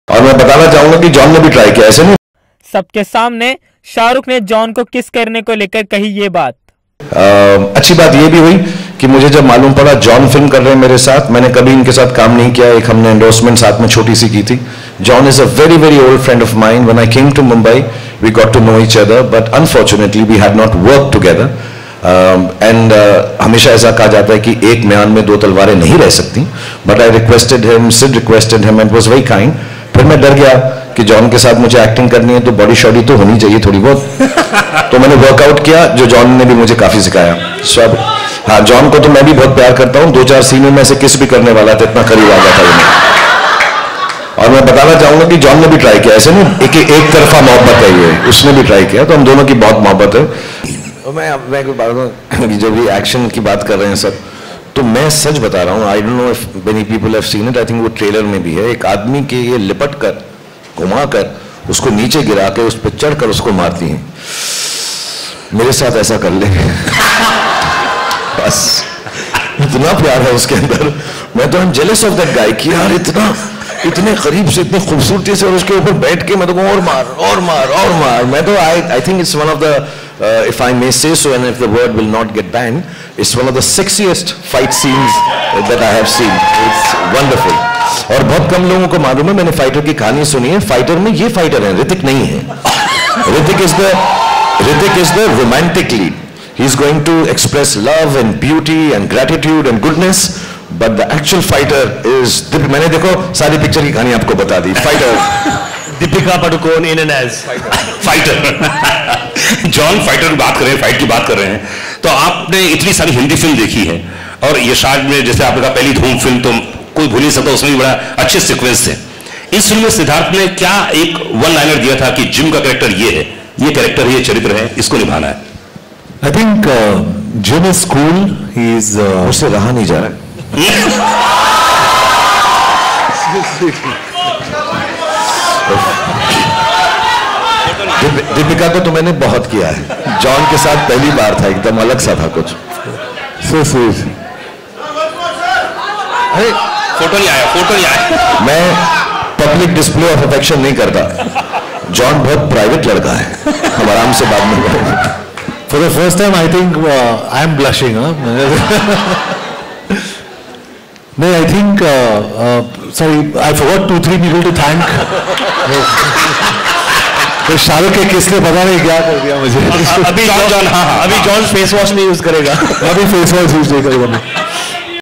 Shahrukh Khan On Kissing John Abraham Deepika Padukone in Pathaan
Welcome to Bollywood Television Channel , your one stop destination for everything and anything around Movies, Celebrities and Indian Television stars.